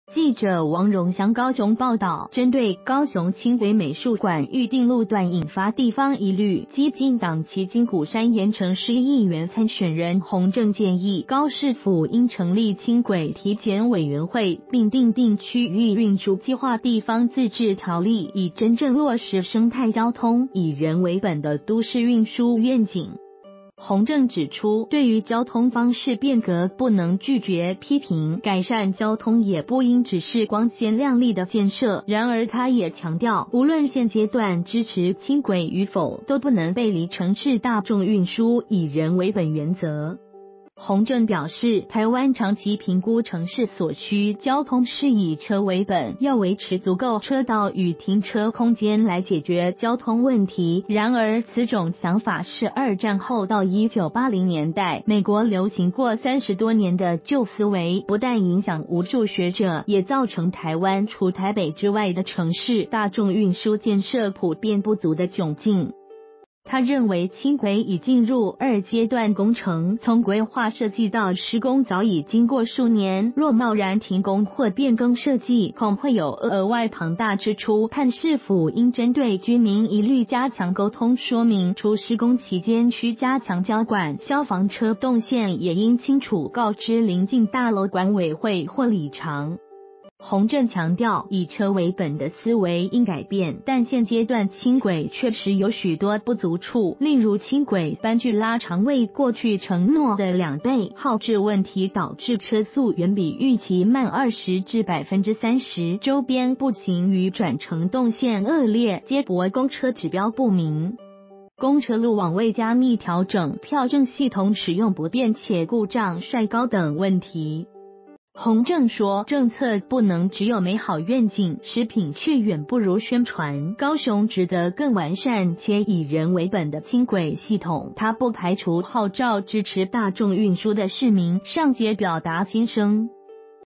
基進黨議員參選人籲高市府 成立輕軌體檢委員會
〔記者王榮祥／高雄報導〕針對高雄輕軌美術館預定路段引發地方疑慮！基進黨旗津鼓山鹽埕市議員參選人洪正建議，高市府應成立「輕軌體檢委員會」並訂定「區域運輸計畫地方自治條例」，以真正落實生態交通、以人為本的都市運輸願景。

發表評論評論如下。